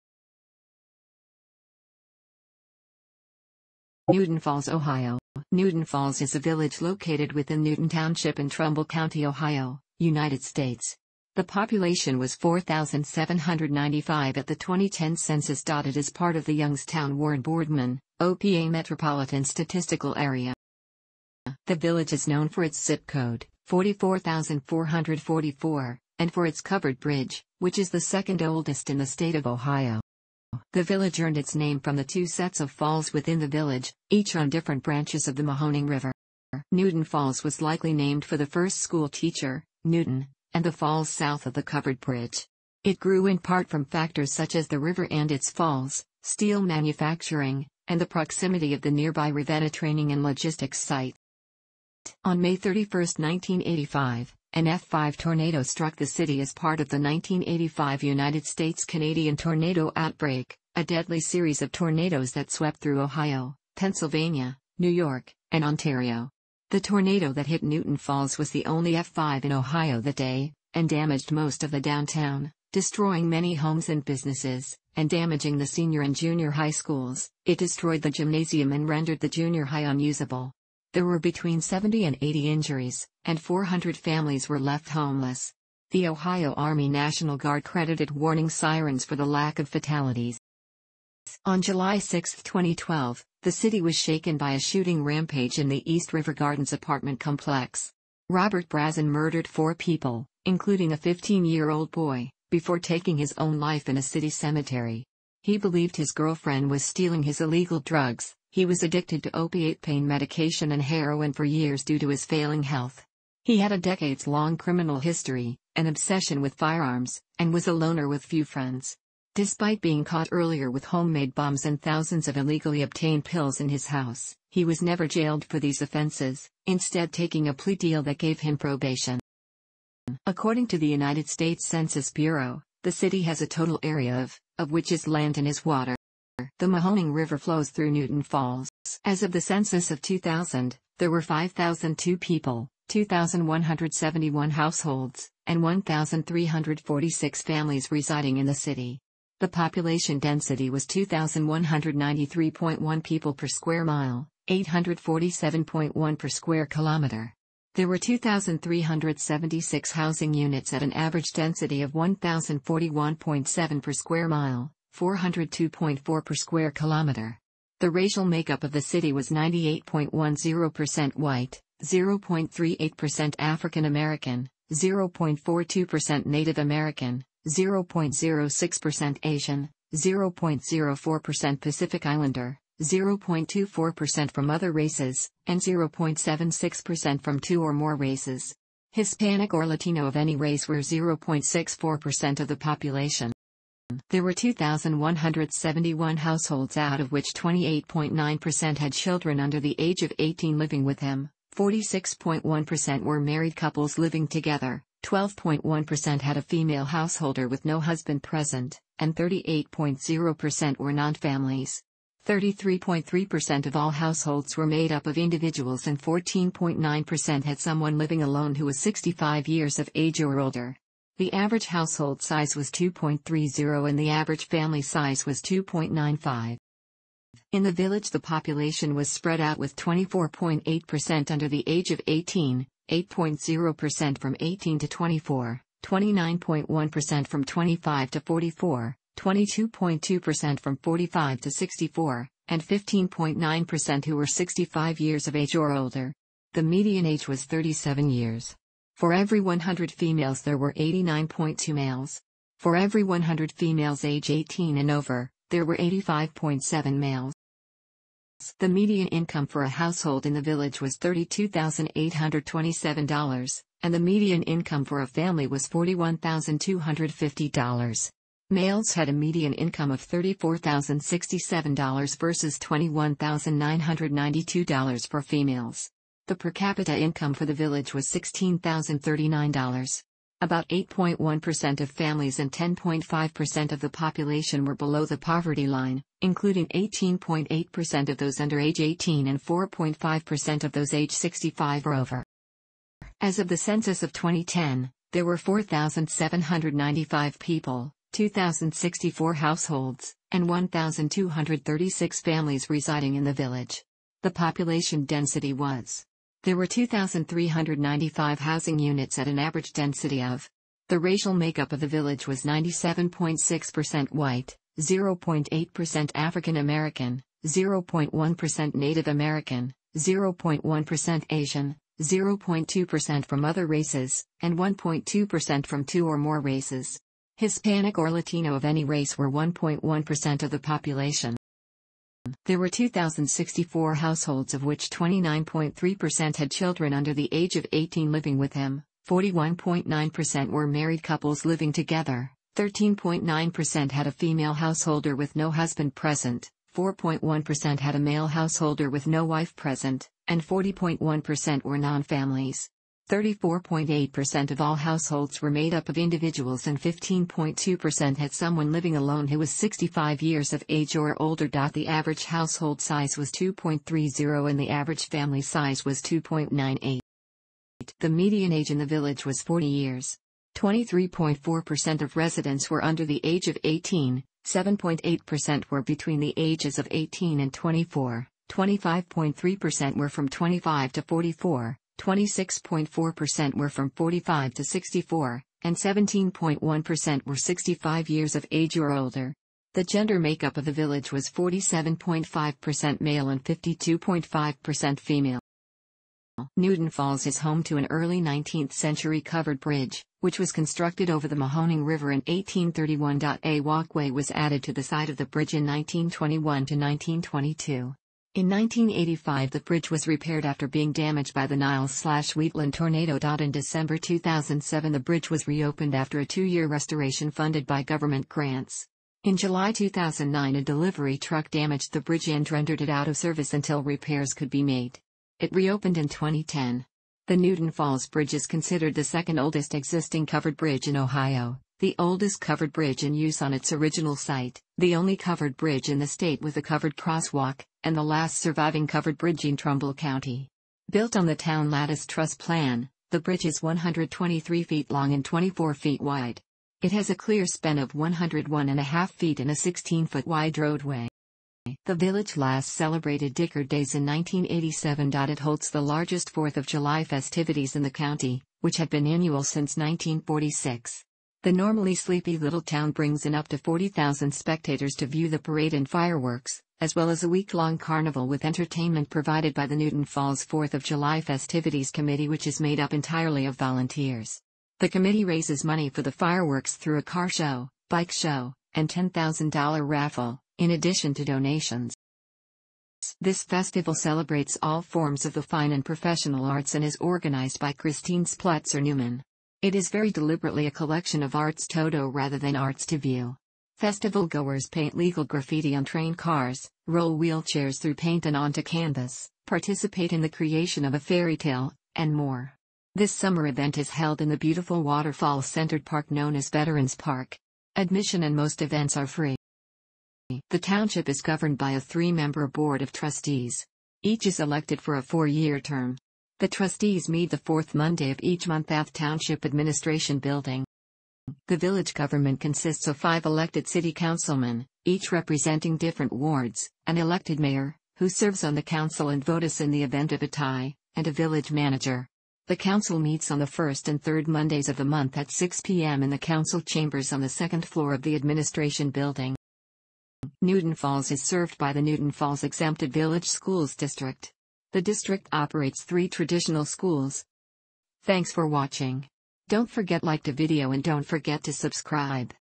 Newton Falls, Ohio
Newton Falls is a village located within Newton Township in Trumbull County, Ohio, United States. The population was 4,795 at the 2010 census. It is part of the Youngstown-Warren-Boardman, OH-PA Metropolitan Statistical Area.

The village is known for its ZIP code (44444) and for its covered bridge, which is the second oldest in the state of Ohio.

The village earned its name from the two sets of falls within the village, each on different branches of the Mahoning River.

Newton Falls was likely named for the first school teacher, Newton, and the falls south of the Covered Bridge. It grew in part from factors such as the river and its falls, steel manufacturing, and the proximity of the nearby Ravenna Training and Logistics Site.

On May 31, 1985, an F5 tornado struck the city as part of the 1985 United States-Canadian tornado outbreak, a deadly series of tornadoes that swept through Ohio, Pennsylvania, New York, and Ontario. The tornado that hit Newton Falls was the only F5 in Ohio that day, and damaged most of the downtown, destroying many homes and businesses, and damaging the senior and junior high schools (it destroyed the gymnasium and rendered the junior high unusable). There were between 70 and 80 injuries, and 400 families were left homeless. The Ohio Army National Guard credited warning sirens for the lack of fatalities.

On July 6, 2012, the city was shaken by a shooting rampage in the East River Gardens apartment complex. Robert Brazzon murdered four people, including a fifteen-year-old boy, before taking his own life in a city cemetery. He believed his girlfriend was stealing his illegal drugs; he was addicted to opiate pain medication and heroin for years due to his failing health. He had a decades-long criminal history, an obsession with firearms, and was a loner with few friends. Despite being caught earlier with homemade bombs and thousand...